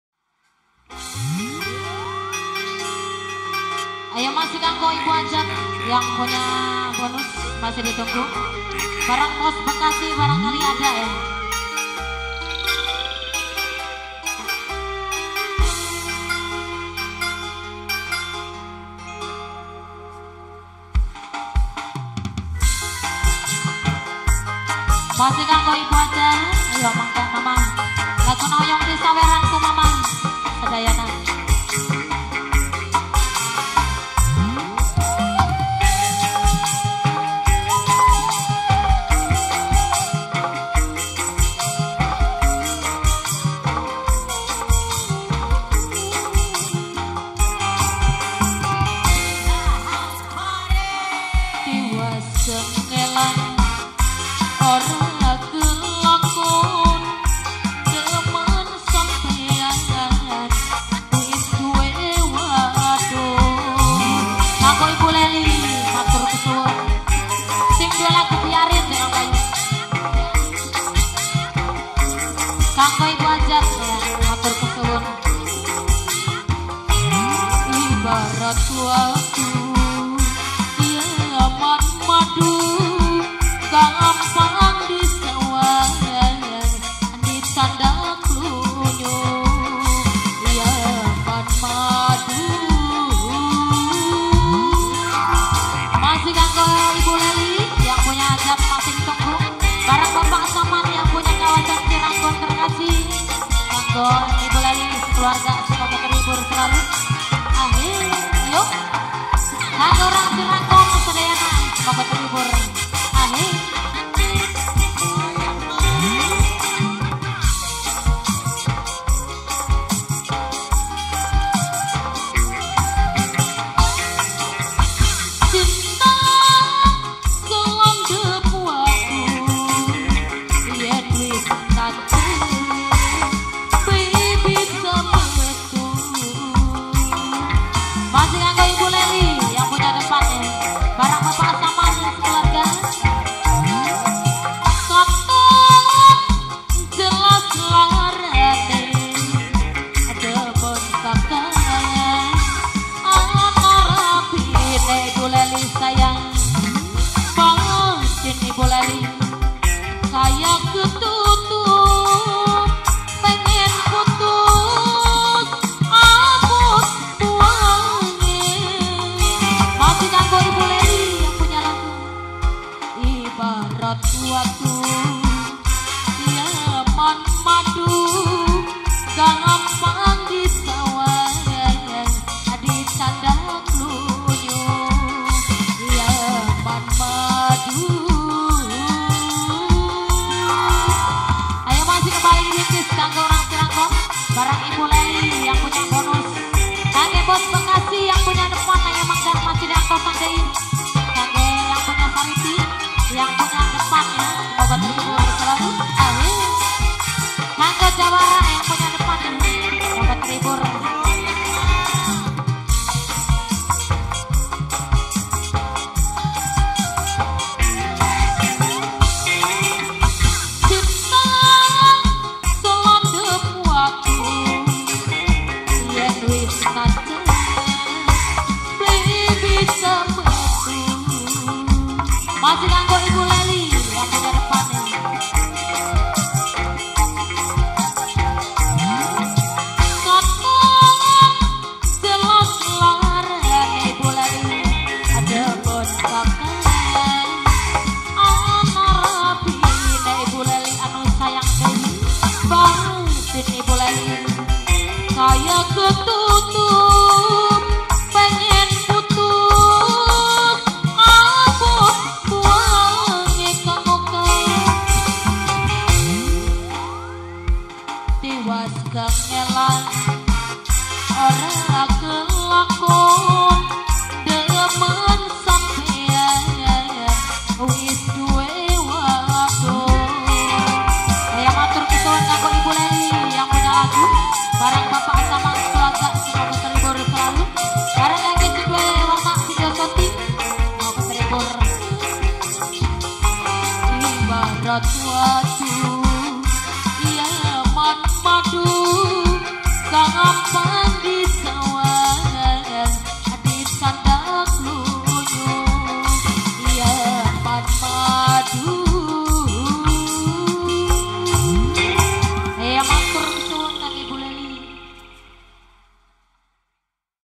YAMAN MADU PUTRA GENADES BPK SA'AMAN WARNA PRO
Singa Dangdut "PUTRA GENADES" dari Anggasari-Subang.
Edisi: Minggu, 10 Januari 2016
Kp. Ciranggon Ds. Rancamulya Kec. Patokbeusi - Subang.
Pesta Perahajatan: Bpk. Sa'aman - Ibu Lely A.Putri.